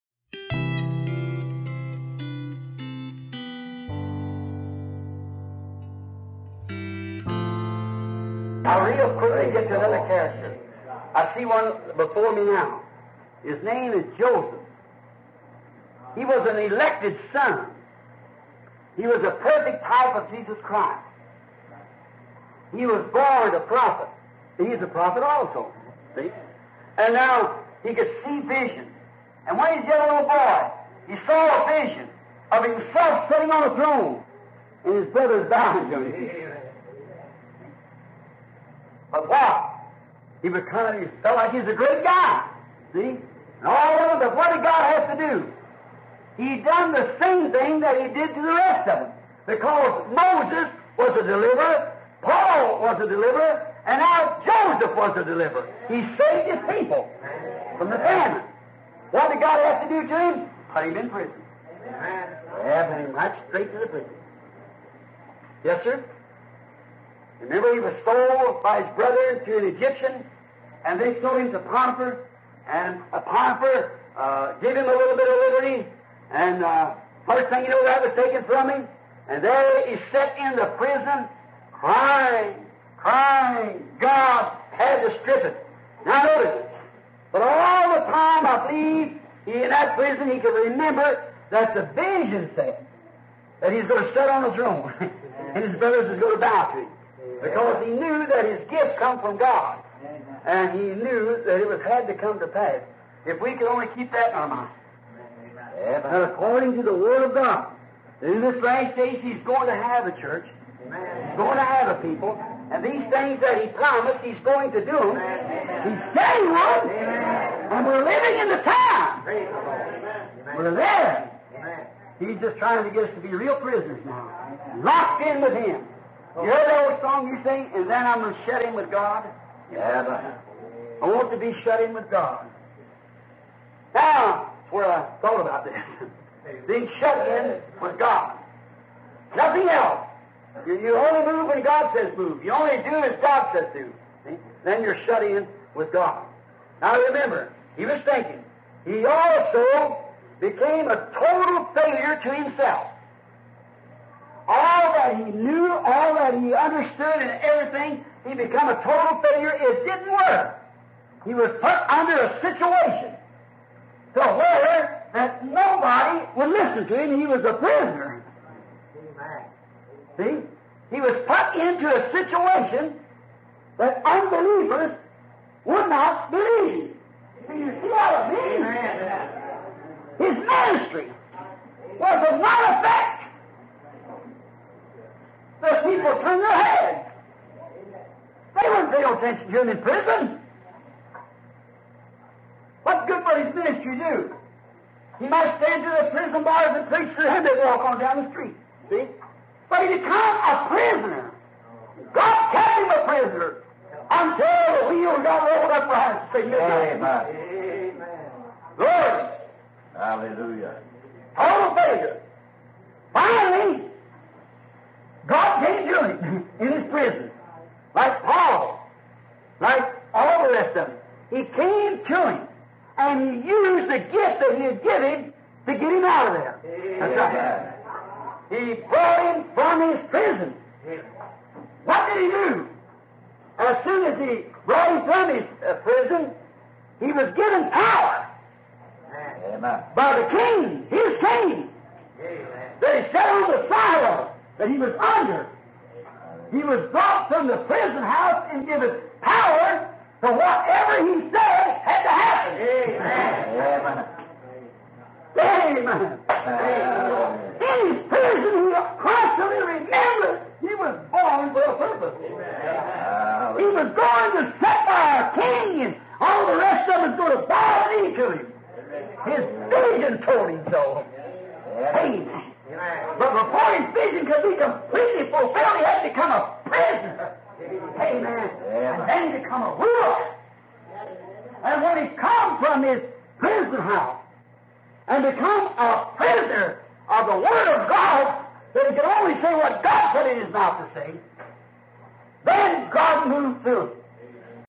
When You Become a Prisoner Of Christ - WILLIAM BRANHAM
167 I’ll real quickly get to another character. I see one before me now. His name is Joseph. He was an elected son. He was a perfect type of Jesus Christ. He was borned a prophet. He was a prophet, also. See? And now he could see vision. And when he’s yet a little boy, he saw a vision of himself sitting on a throne, and his brothers bowing to him. See? But watch. He become…He felt like he was a great guy. See? And all of them…

168 But what did God have to do? He done the same thing that He did to the rest of them. Because, Moses was a deliverer, Paul was a deliverer, and now Joseph was a deliverer. He saved his people from the famine.